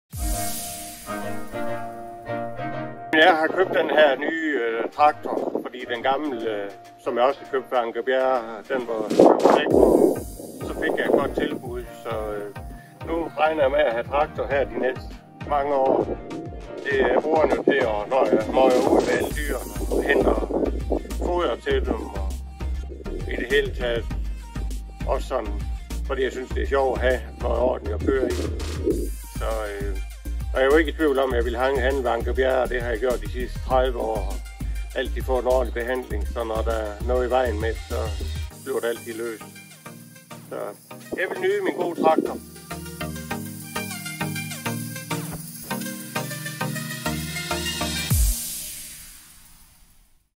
New Holland Boomer 55 hos Jørgens Dyr
Æsler, lamaer og en ny New Holland Boomer 55 - så spiller det bare! Hos Jørgens Dyr er der liv og glade dage, og ikke mindst, når Jørgen selv kommer ud på marken, for så ved dyrene godt, at de skal forkæles med foder og kærlighed.  Dyrene spiller en stor rolle i Jørgens liv - og for nogle af dyrene har de også en rolle i diverse film- og reklameproduktioner, så hvis du kommer vejen forbi Jørgens Dyr, kan du muligvis spotte en kendis ;-)

Jørgen har været en loyal kunde hos Anker Bjerre A/S i flere årtier - og det var derfor også helt naturligt for ham at handle kompakttraktor hos os igen. Denne gang har Jørgen købt en splinterny Boomer 55, som skal bruges til forskellige praktiske gøremål som f.eks. til hentning af foder.
Se dronevideoen - og hør Jørgen fortælle om traktoren med egne ord.

Kontakt Anker Bjerre A/S for yderligere info på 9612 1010 (Holstebro) og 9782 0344 (Lemvig)